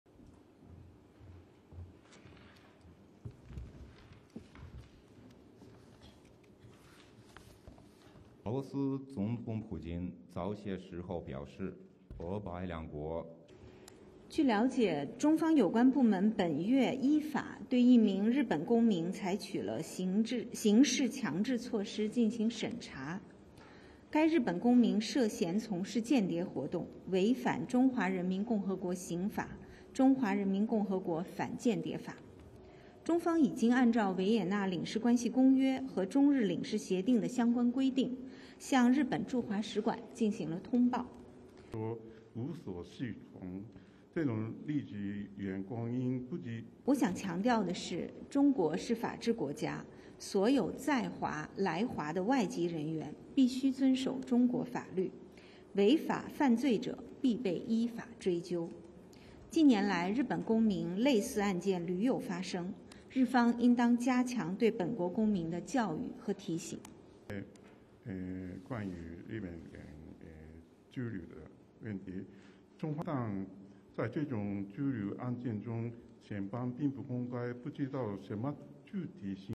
China confirms detention of Japanese citizen
RESTRICTION SUMMARY:

ASSOCIATED PRESS
Beijing -  27 March 2023
1. Track of Mao Ning, spokesperson of Foreign Ministry, walking to podium
2. Cutaway of reporters
3. SOUNDBITE (Mandarin) Mao Ning, spokesperson, Ministry of Foreign Affairs:
“It is learned that relevant Chinese authorities have adopted compulsory measures in accordance with the criminal procedure law against a Japanese citizen for a case examination this month. The Japanese citizen is suspected of engaging in espionage activities in violation of the Criminal Law of the People’s Republic of China and the Anti-espionage Law of the People’s Republic of China. China has notified the Japanese embassy in China in accordance with the relevant provisions of the Vienna Convention on Consular Relations and the China-Japan Consular Agreement.”
4. Wide of briefing
5. SOUNDBITE (Mandarin) Mao Ning, spokesperson, Ministry of Foreign Affairs:
“What I want to emphasize is that China is a country of rule of law. All foreign nationals who come to China and live in China must abide by Chinese laws, and those who break the law and commit crimes must be held accountable according to the law. In recent years, similar cases of Japanese citizens have occurred repeatedly. The Japanese side should strengthen education among its citizens and send them reminders.”
6. Cutaway of reporters
7. Wide of briefing
STORYLINE:
China’s foreign ministry confirmed on Monday the detainment of a Japanese citizen over suspected espionage.

Foreign ministry spokesperson Mao Ning said authorities detained a Japanese citizen this month suspected of engaging in espionage activities.

Mao said China has informed the Japanese embassy in China.

A Japanese pharmaceutical company Astellas Pharma Inc. acknowledged that the man detained in Beijing is an employee of the firm, but declined to disclose further details, including his name, position and whether he is based in China.

The company said it was seeking information from the Japanese Foreign Ministry.

Tokyo has demanded that China release a Japanese national who was detained in Beijing earlier this month, Japan’s top government spokesperson said Monday.

The Japanese government has since demanded the early release of the Japanese national.

Japan also requested that Chinese authorities allow the man access to Japanese consulate officials.

AP video shot by Liu Zheng